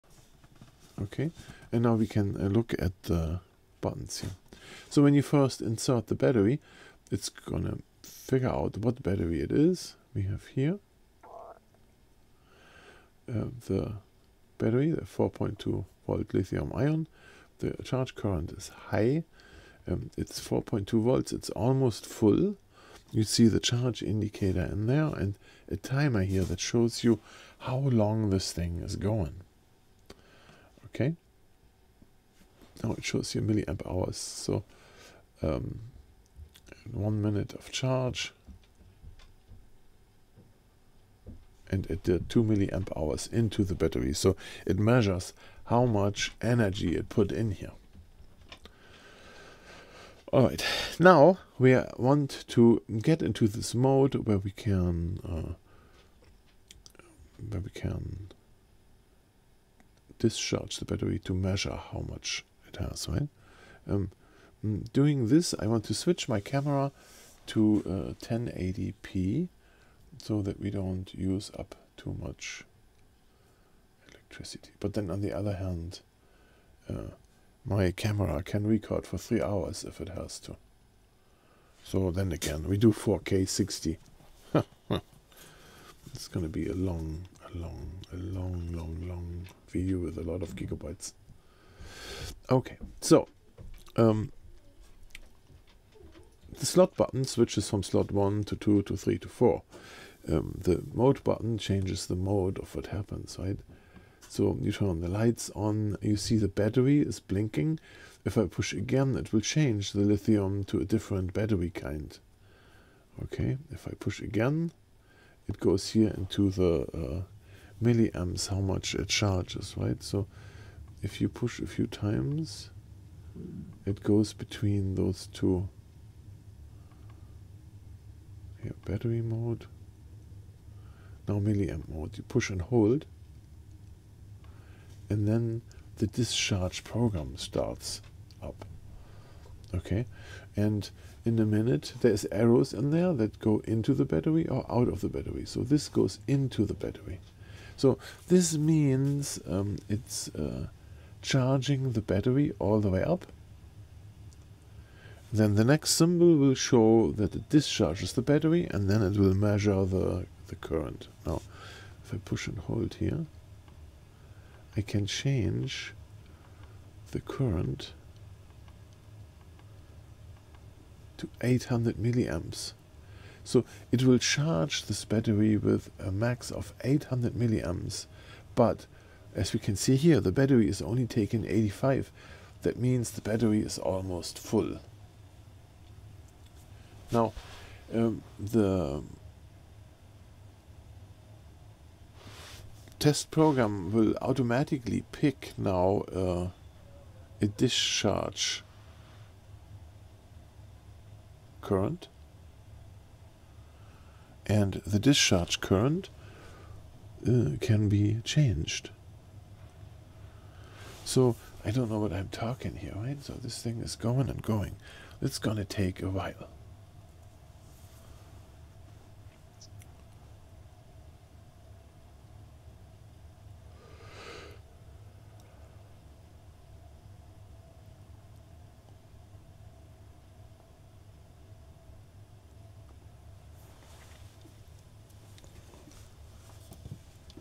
How to measure Capacity with the MiBOXER C4 Charger
This video shows how to measure the capacity of a rechargeable battery.
There are videos out here on YouTube that say that it doesn't have the capability, but those guys probably didn't read the manual.
I show how it is done. One thing to keep in mind. It takes a while to measure, because this charger is discharging with max 400mA, and when you divide your batteries capacity by 400, then you'll get the amount of hour that a discharge process takes. After it is discharged the charger will charge the battery with max 800mA. If you take your batteries capacity, and divide it by 800, then you'll get the amount of hour the charge process is going to take.
Make sure to always read and follow the safety advice in the manual before you use a Li-Ion charger!